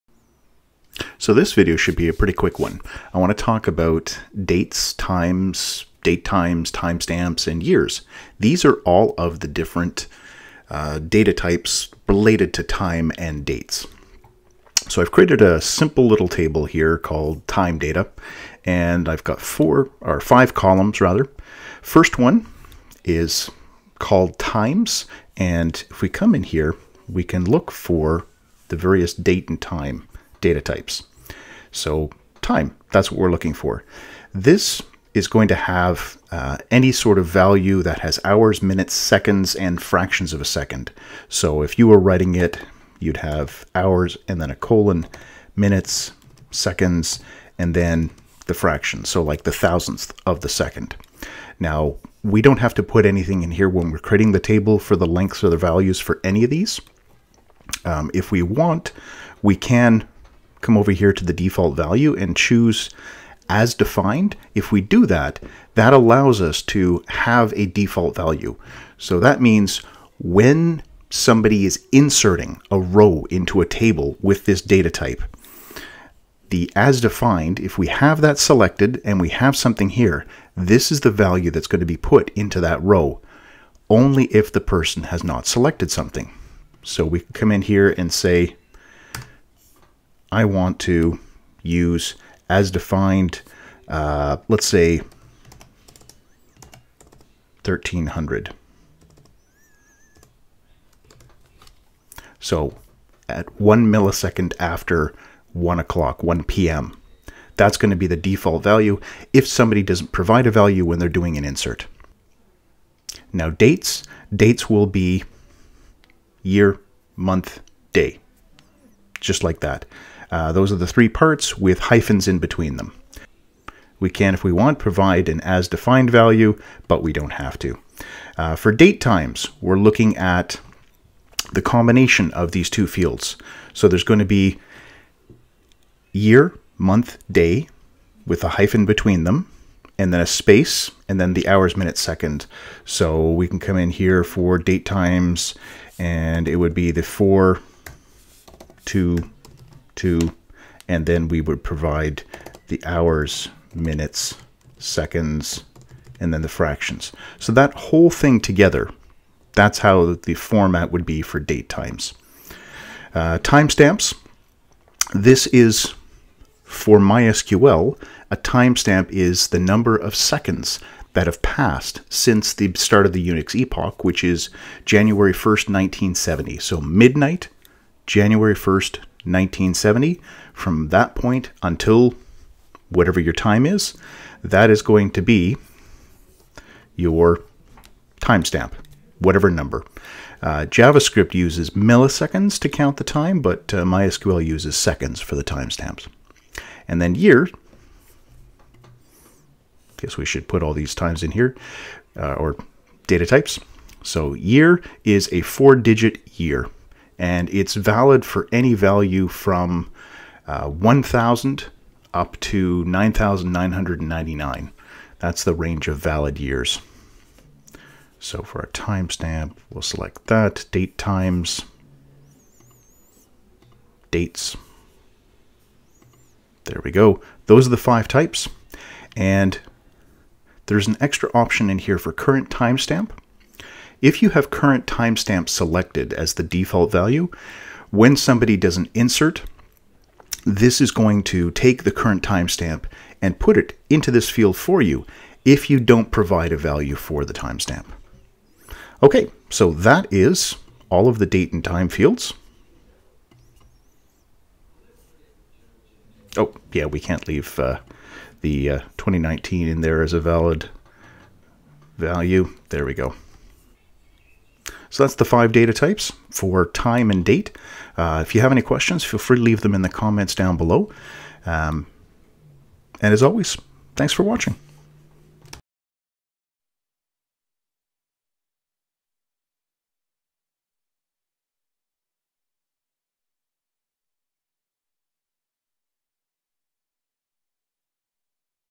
Learning MySQL - DataTypes for Time and Dates
This tutorial covers the different data types that are available in MySQL for dates and times.
current timestamp default values are also discussed.